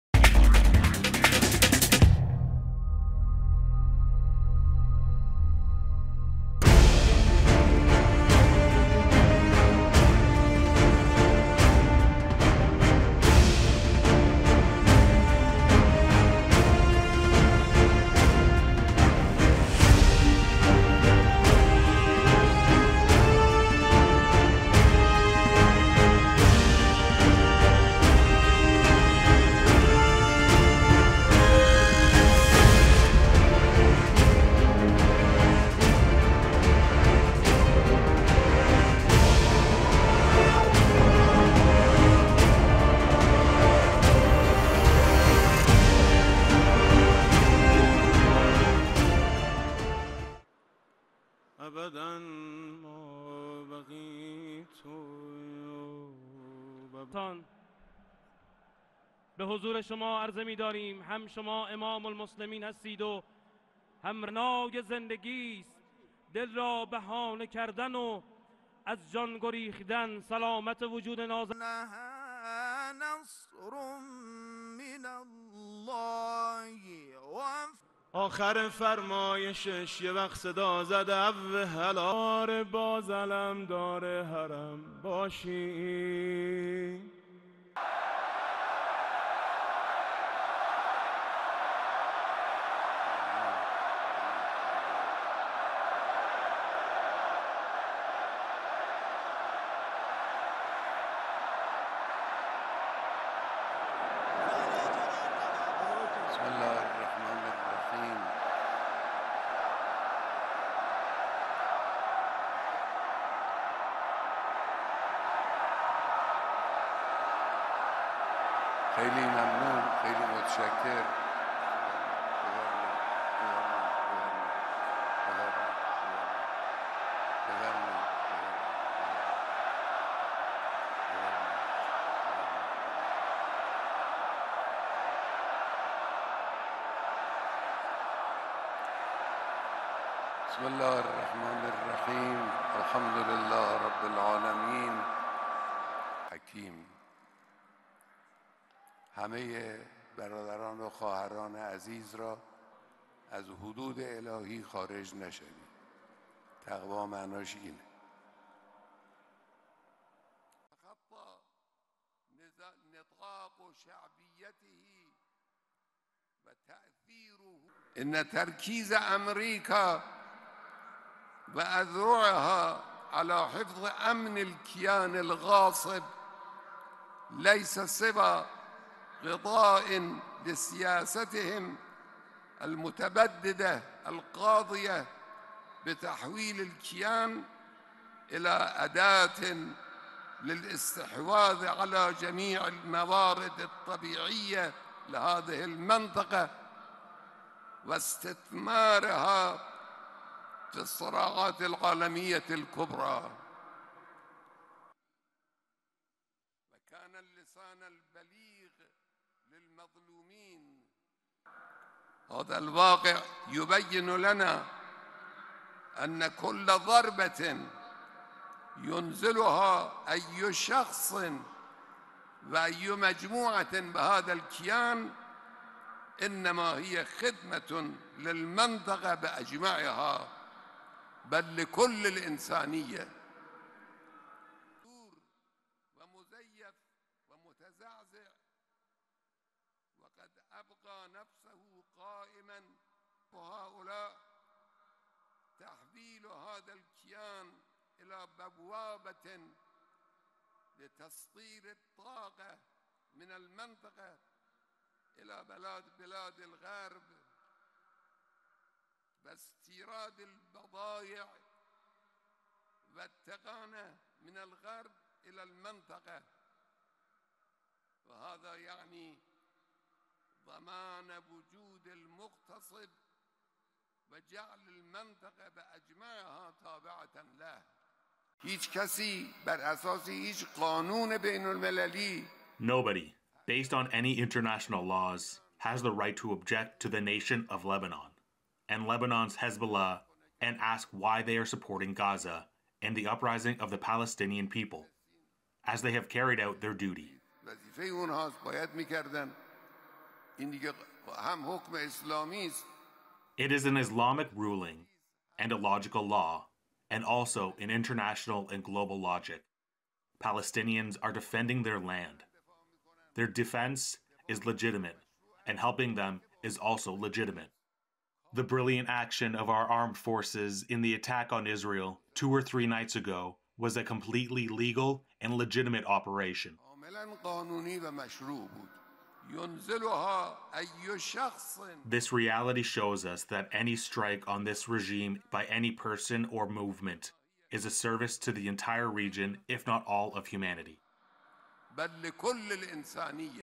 Israel Iran War News Today | Iran's Supreme Leader Ayatollah Ali Khamenei Warns Israel | N18G
Ayatollah Ali Khamenei, Iran’s supreme leader, delivered a defiant sermon at Friday prayers, in which he said Iran’s strikes against Israel this week were “totally legal and legitimate.” It’s rare for Khamenei to lead Friday prayers — the last time he did so was at another moment of national crisis, after the killing of Qasem Soleimani in a U.S. drone strike in 2020. Israeli strikes cut off a key border crossing between Lebanon and Syria as the Israel Defense Forces escalated its offensive with overnight strikes in Beirut’s southern suburbs and expanded evacuation orders in southern Lebanon.